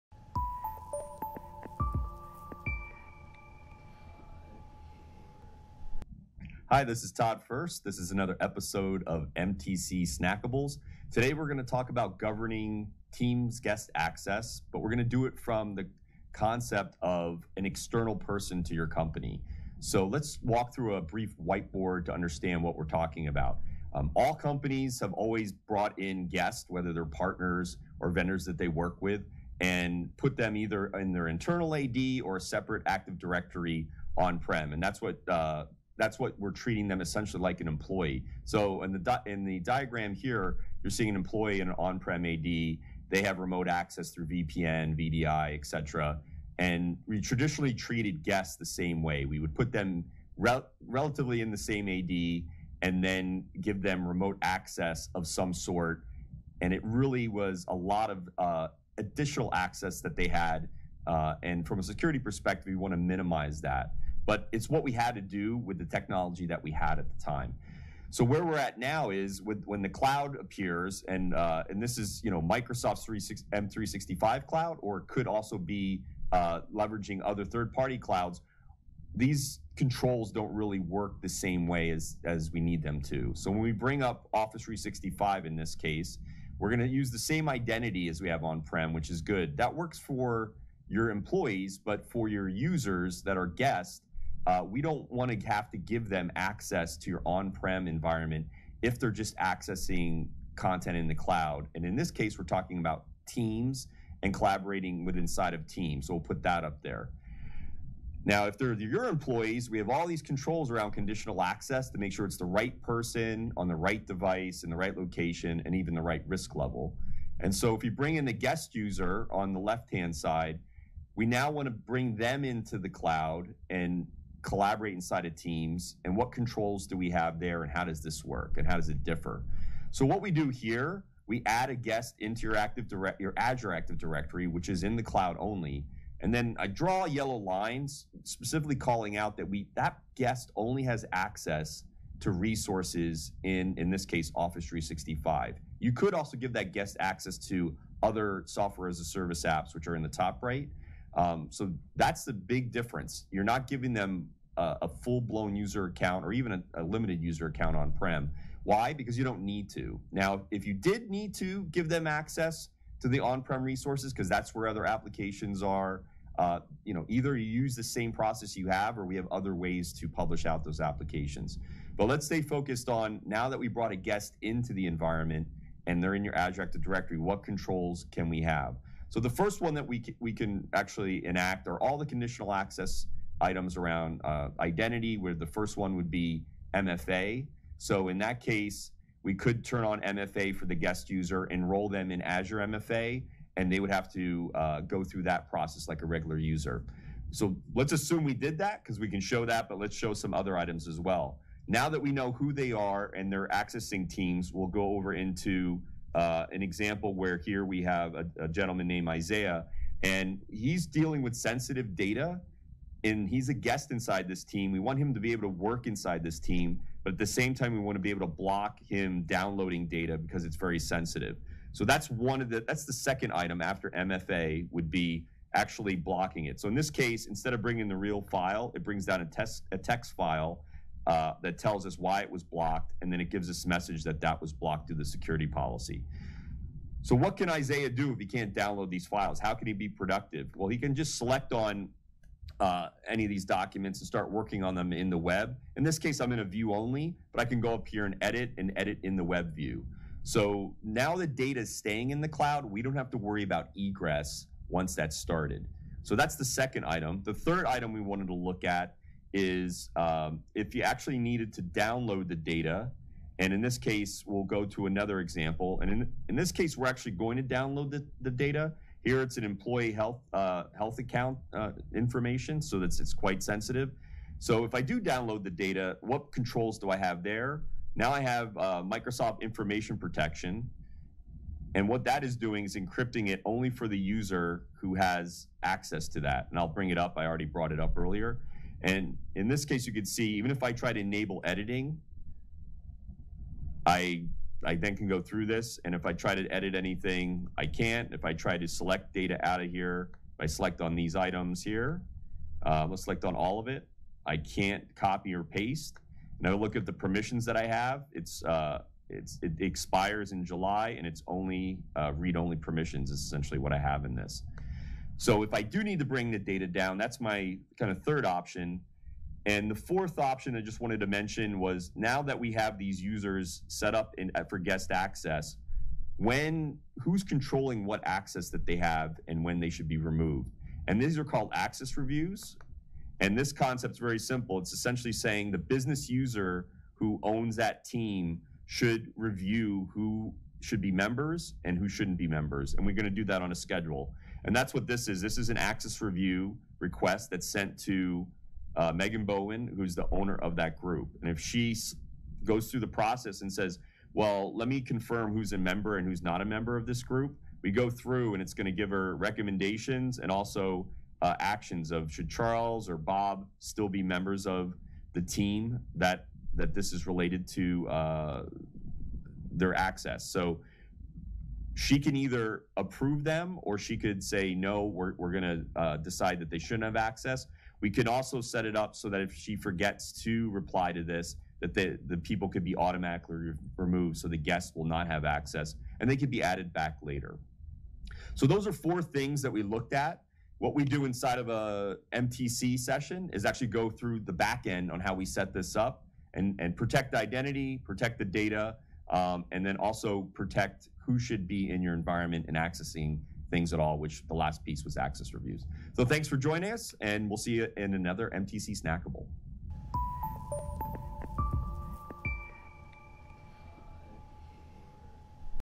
MTC Snackables - Microsoft Teams Guest Access: Security and Governance
In this episode of MTC Snackables, Philly's Chief Technical Architect talks about Guest Access in Microsoft Teams.